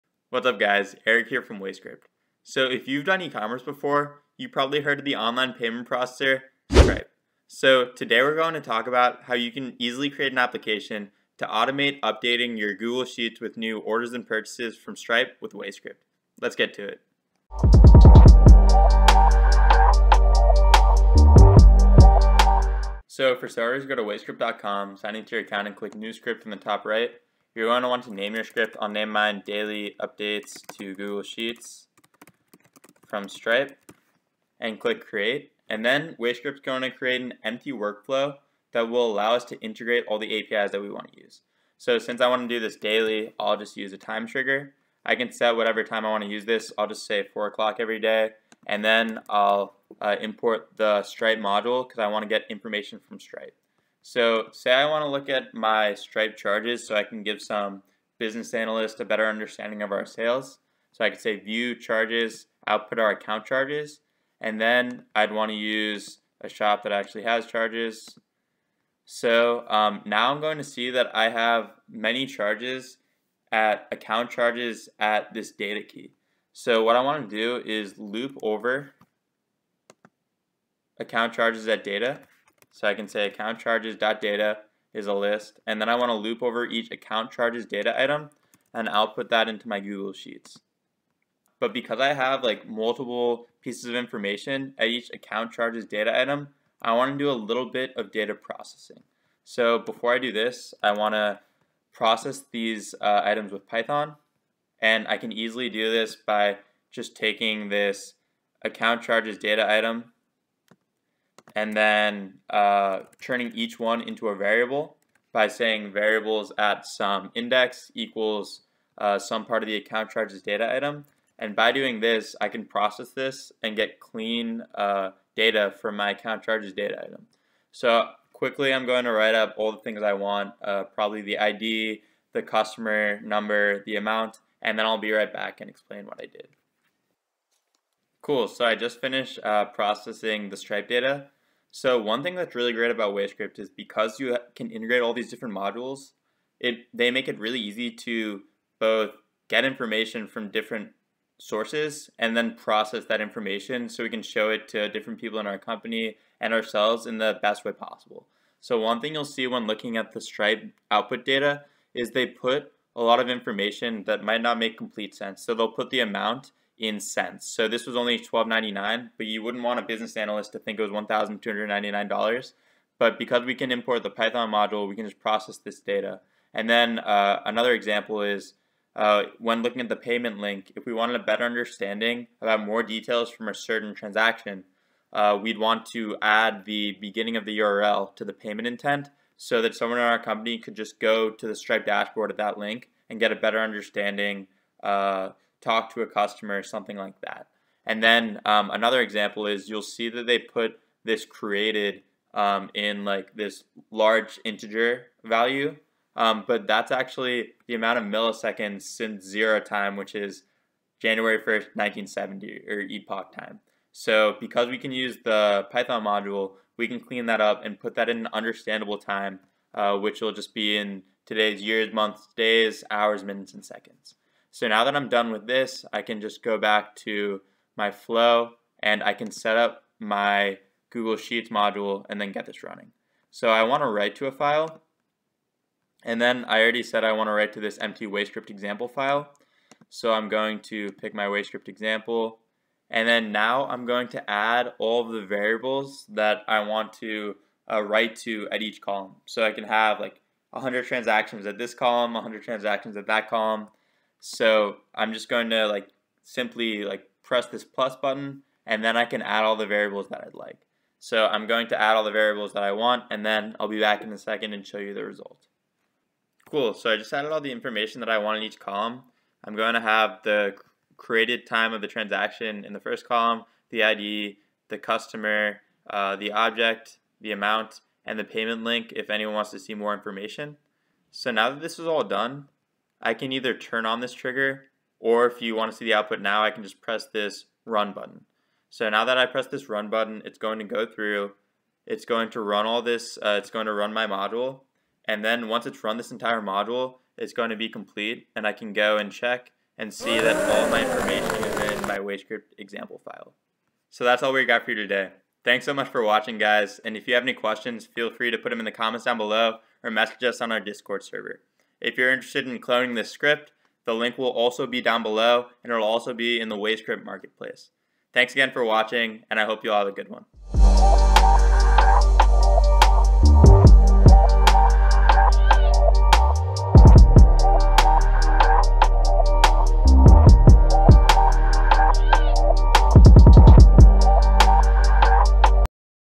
Write Stripe Data to Google Sheets Automatically | WayScript for Business
Hey everyone, in this one we're taking data from our stripe accounts and programmatically adding them to google sheets.

Questions about this script or anything else? We're always around to help.